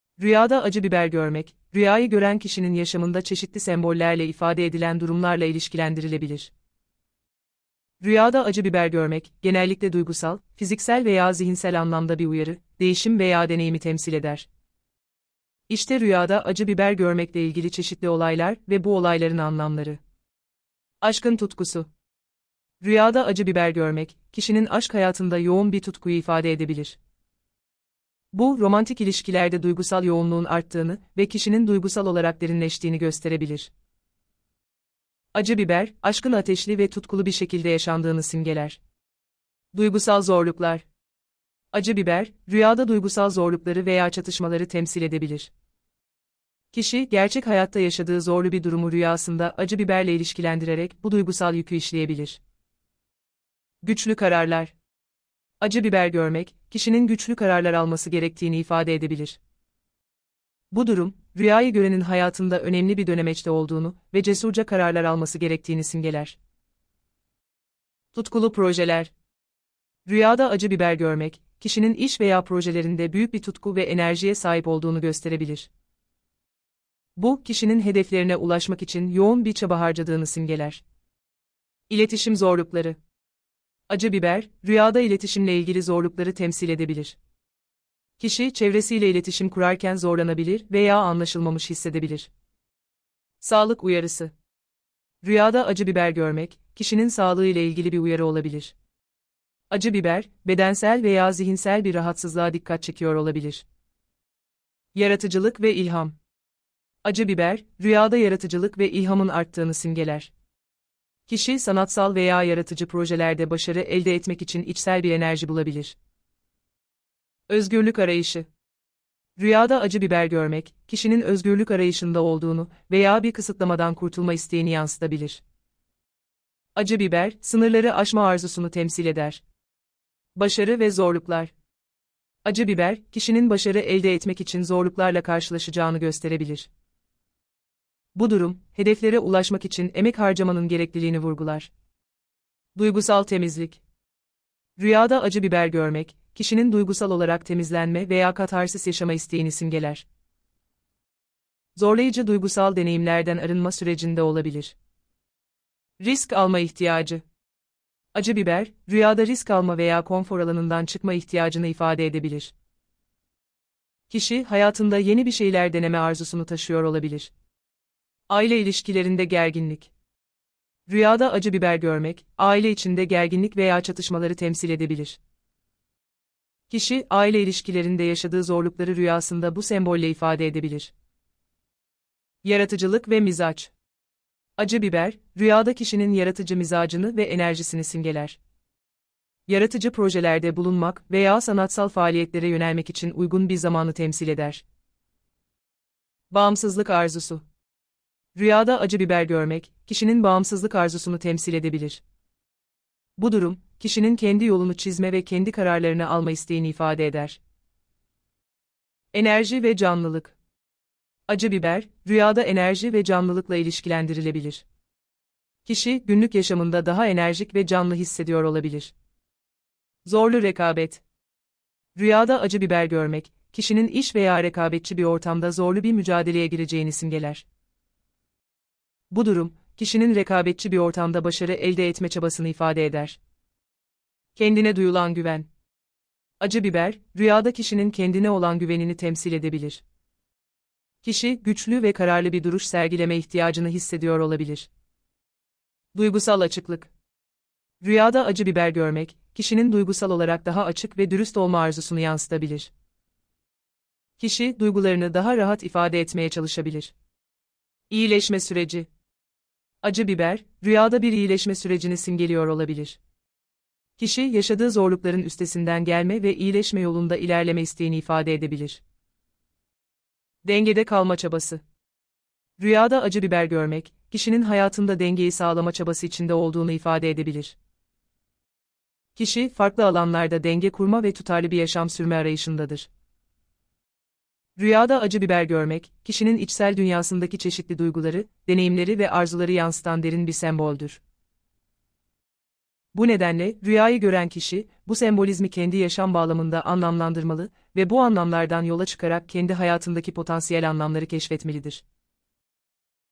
Rüyada Acı Biber Görmek
Rüyada acı biber görmek, rüyayı gören kişinin yaşamında çeşitli sembollerle ifade edilen durumlarla ilişkilendirilebilir. Rüyada acı biber görmek, genellikle duygusal, fiziksel veya zihinsel anlamda bir uyarı, değişim veya deneyimi temsil eder. İşte rüyada acı biber görmekle ilgili çeşitli olaylar ve bu olayların anlamları:

Aşkın Tutkusu:
Rüyada acı biber görmek, kişinin aşk hayatında yoğun bir tutkuyu ifade edebilir. Bu, romantik ilişkilerde duygusal yoğunluğun arttığını ve kişinin duygusal olarak derinleştiğini gösterebilir. Acı biber, aşkın ateşli ve tutkulu bir şekilde yaşandığını simgeler.

Duygusal Zorluklar:
Acı biber, rüyada duygusal zorlukları veya çatışmaları temsil edebilir. Kişi, gerçek hayatta yaşadığı zorlu bir durumu rüyasında acı biberle ilişkilendirerek bu duygusal yükü işleyebilir.

Güçlü Kararlar:
Acı biber görmek, kişinin güçlü kararlar alması gerektiğini ifade edebilir. Bu durum, rüyayı görenin hayatında önemli bir dönemeçte olduğunu ve cesurca kararlar alması gerektiğini simgeler.

Tutkulu Projeler:
Rüyada acı biber görmek, kişinin iş veya projelerinde büyük bir tutku ve enerjiye sahip olduğunu gösterebilir. Bu, kişinin hedeflerine ulaşmak için yoğun bir çaba harcadığını simgeler.

Sağlık Uyarısı:
Rüyada acı biber görmek, kişinin sağlığı ile ilgili bir uyarı olabilir. Acı biber, bedensel veya zihinsel bir rahatsızlığa dikkat çekiyor olabilir.

Yaratıcılık ve İlham:
Acı biber, rüyada yaratıcılık ve ilhamın arttığını simgeler. Kişi, sanatsal veya yaratıcı projelerde başarı elde etmek için içsel bir enerji bulabilir.

Özgürlük Arayışı:
Rüyada acı biber görmek, kişinin özgürlük arayışında olduğunu veya bir kısıtlamadan kurtulma isteğini yansıtabilir. Acı biber, sınırları aşma arzusunu temsil eder.

Başarı ve Zorluklar:
Acı biber, kişinin başarı elde etmek için zorluklarla karşılaşacağını gösterebilir. Bu durum, hedeflere ulaşmak için emek harcamanın gerekliliğini vurgular.

Duygusal Temizlik:
Rüyada acı biber görmek, kişinin duygusal olarak temizlenme veya katharsis yaşama isteğini simgeler. Zorlayıcı duygusal deneyimlerden arınma sürecinde olabilir.

Risk Alma İhtiyacı:
Acı biber, rüyada risk alma veya konfor alanından çıkma ihtiyacını ifade edebilir. Kişi, hayatında yeni bir şeyler deneme arzusunu taşıyor olabilir.

Aile İlişkilerinde Gerginlik:
Rüyada acı biber görmek, aile içinde gerginlik veya çatışmaları temsil edebilir. Kişi, aile ilişkilerinde yaşadığı zorlukları rüyasında bu sembolle ifade edebilir.

Yaratıcılık ve Mizaç:
Acı biber, rüyada kişinin yaratıcı mizacını ve enerjisini simgeler. Yaratıcı projelerde bulunmak veya sanatsal faaliyetlere yönelmek için uygun bir zamanı temsil eder.

Bağımsızlık Arzusu:
Rüyada acı biber görmek, kişinin bağımsızlık arzusunu temsil edebilir. Bu durum, kişinin kendi yolunu çizme ve kendi kararlarını alma isteğini ifade eder.

Enerji ve Canlılık:
Acı biber, rüyada enerji ve canlılıkla ilişkilendirilebilir. Kişi, günlük yaşamında daha enerjik ve canlı hissediyor olabilir.

Zorlu Rekabet:
Rüyada acı biber görmek, kişinin iş veya rekabetçi bir ortamda zorlu bir mücadeleye gireceğini simgeler. Bu durum, kişinin rekabetçi bir ortamda başarı elde etme çabasını ifade eder.

Kendine Duyulan Güven:
Acı biber, rüyada kişinin kendine olan güvenini temsil edebilir. Kişi, güçlü ve kararlı bir duruş sergileme ihtiyacını hissediyor olabilir.

Duygusal Açıklık:
Rüyada acı biber görmek, kişinin duygusal olarak daha açık ve dürüst olma arzusunu yansıtabilir. Kişi, duygularını daha rahat ifade etmeye çalışabilir.

İyileşme Süreci:
Acı biber, rüyada bir iyileşme sürecini simgeliyor olabilir. Kişi, yaşadığı zorlukların üstesinden gelme ve iyileşme yolunda ilerleme isteğini ifade edebilir.

Dengede Kalma Çabası:
Rüyada acı biber görmek, kişinin hayatında dengeyi sağlama çabası içinde olduğunu ifade edebilir. Kişi, farklı alanlarda denge kurma ve tutarlı bir yaşam sürme arayışındadır.

Rüyada acı biber görmek, kişinin içsel dünyasındaki çeşitli duyguları, deneyimleri ve arzuları yansıtan derin bir semboldür. Bu nedenle, rüyayı gören kişi, bu sembolizmi kendi yaşam bağlamında anlamlandırmalı ve bu anlamlardan yola çıkarak kendi hayatındaki potansiyel anlamları keşfetmelidir.